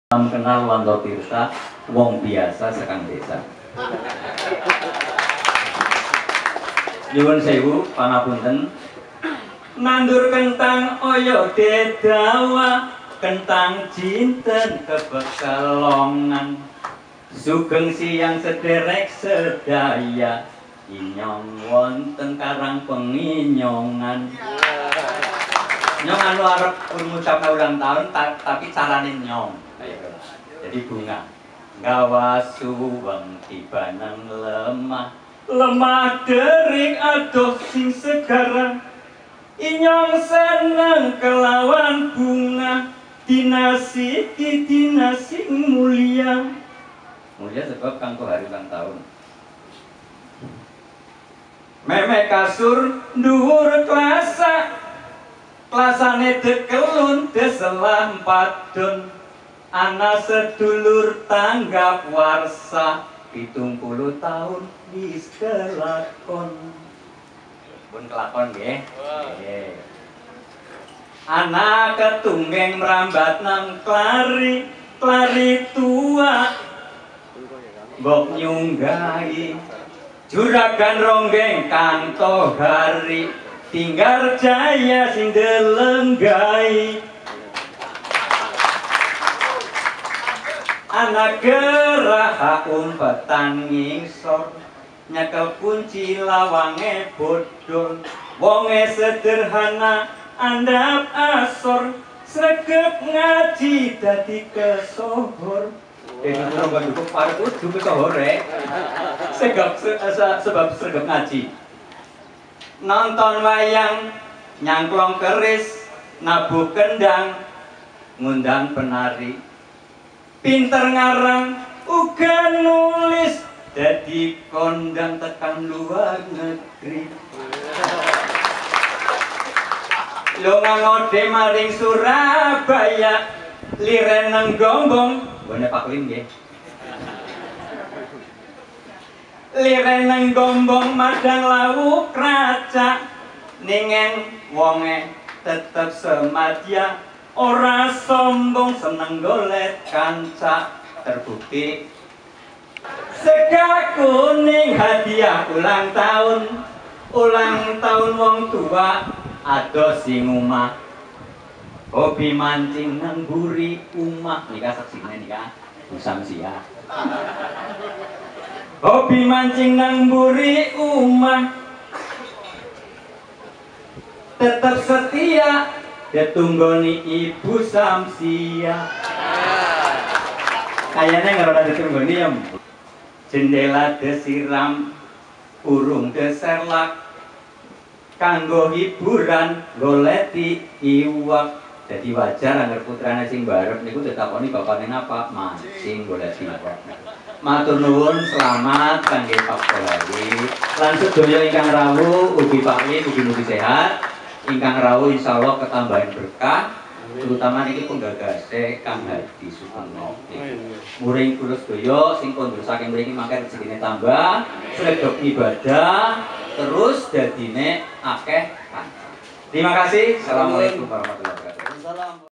PARIKAN (PANTUN BANYUMASAN) WANTO TIRTA PADA ULTAH KE-70 AHMAD TOHARI
Wanto Tirta membawakan parikan pada ulang tahun ke-70 Ahmad Tohari tahun 2018. Parikan adalah pantun dalam bahasa Banyumas